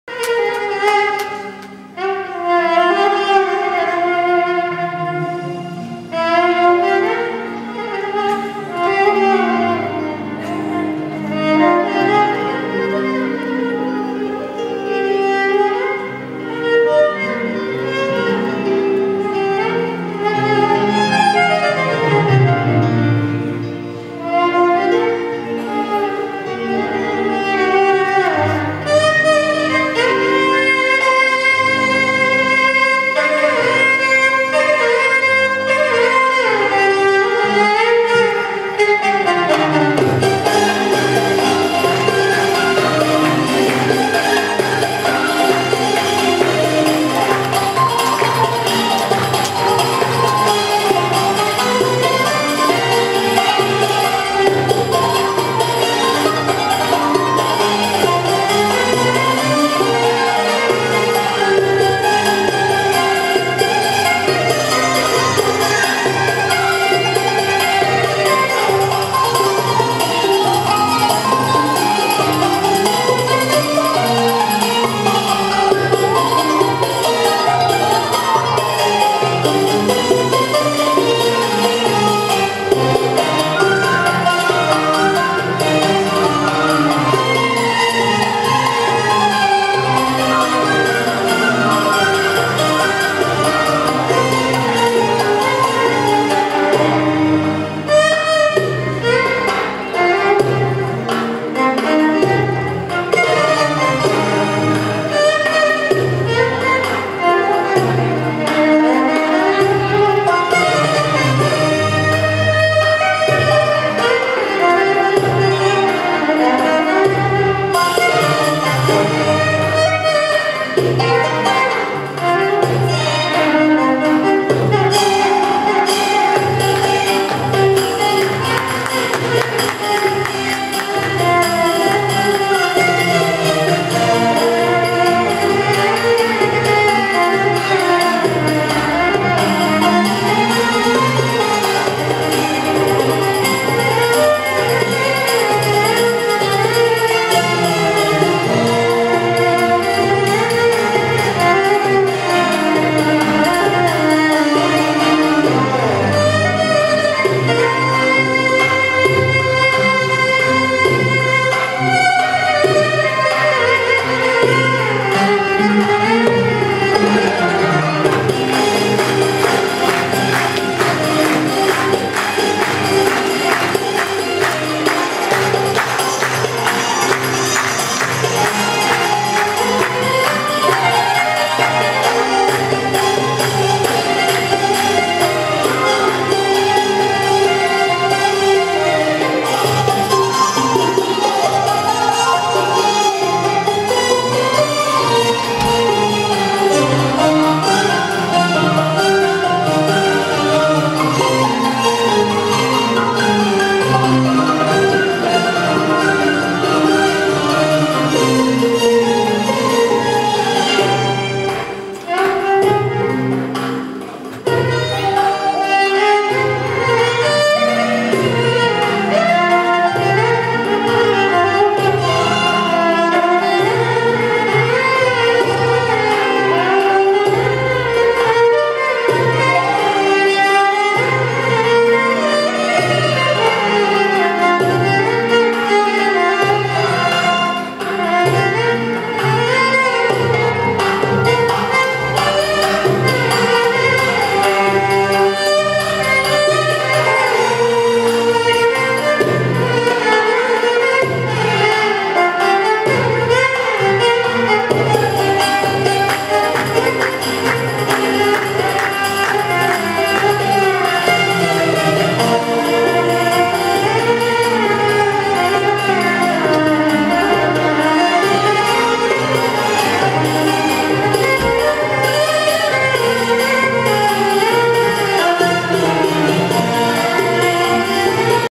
El Hefny group.mov
Concert from the Alexandria Opera Arabic  Music Festival
playing with Jehad Akil